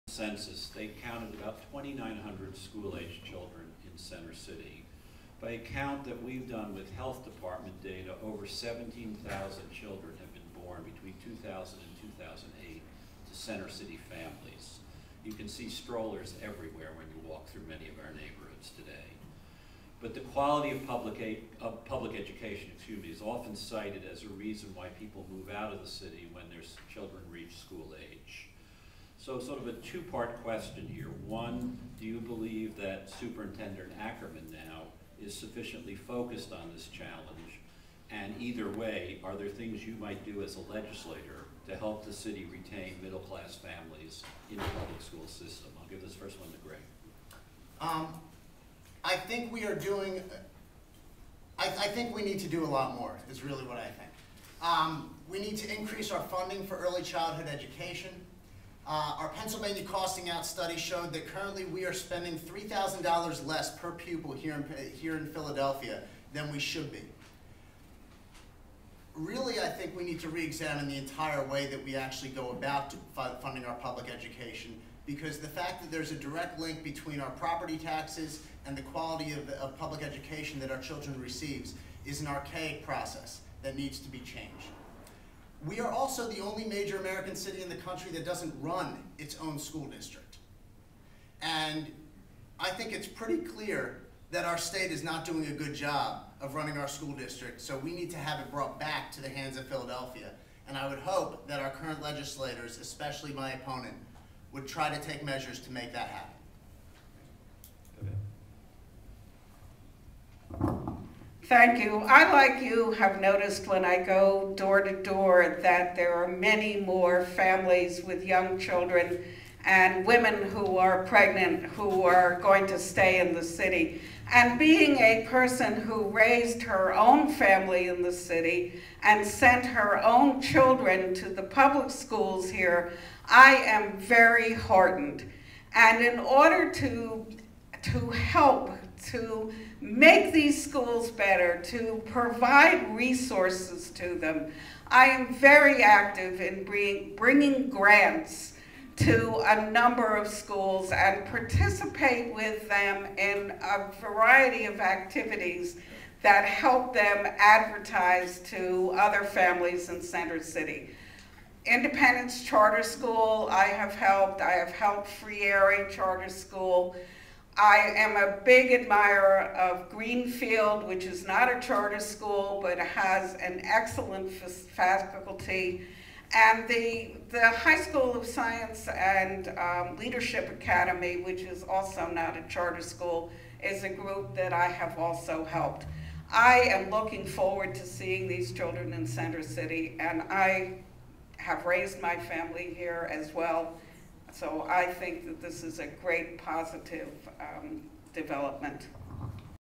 Josephs-Kravitz Debate Part 5
Babette Josephs and Gregg Kravitz discuss the issues